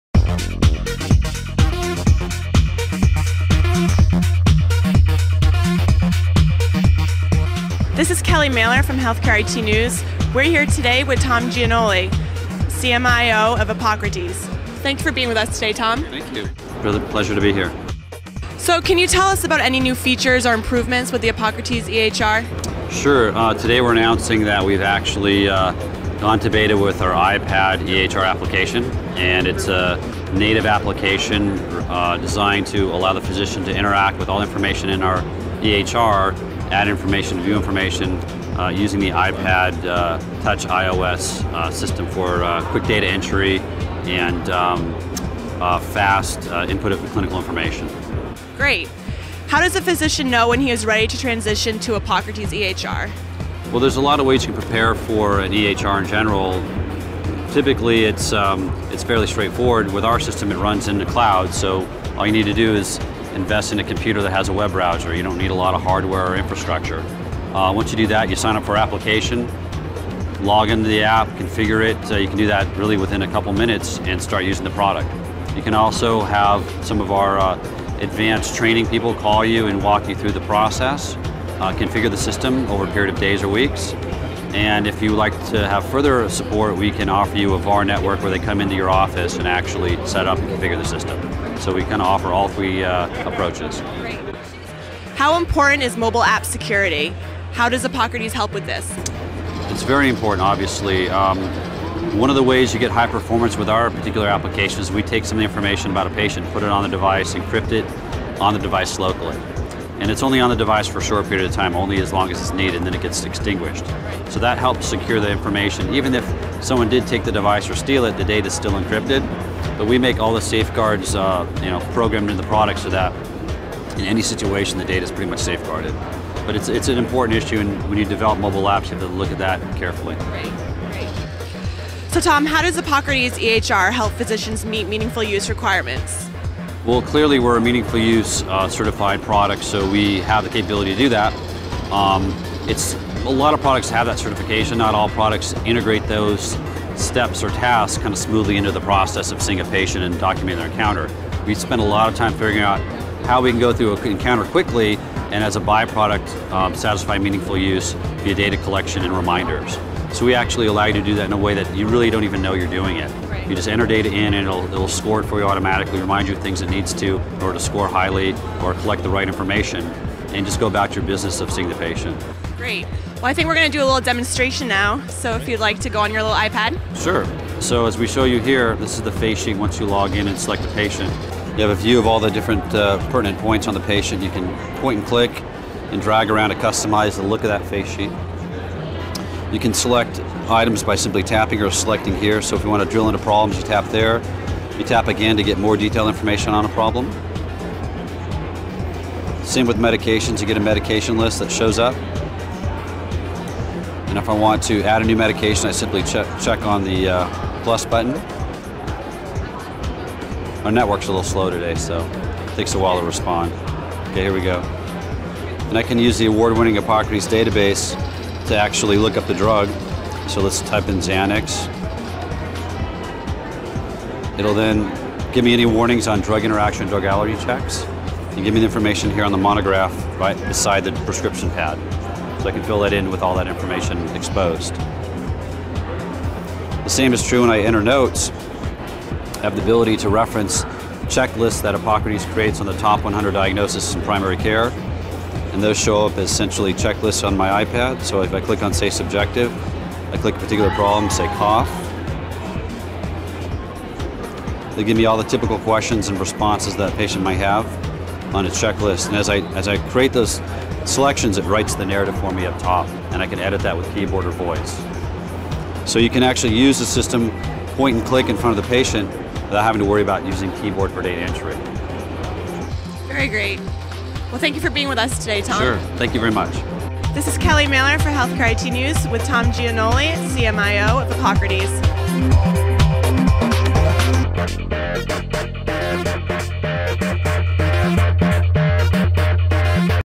Epocrates iPad EHR at HIMSS12
UPDATE: On March 6, 2012, Epocrates announced they will be abandoning this EHR initiative.

We sit down with Tom Giannulli, MD, CMIO at Epocrates, to demo Epocrates' new iPad EHR, one of only a handful of dedicated iPad EHR offerings at HIMSS12. Dr Giannuli walks us through some of the EHR screens, explains how physician practices can begin using the EHR, and tells us how the Epocrates EHR will help physicians meet meaningful use requirements.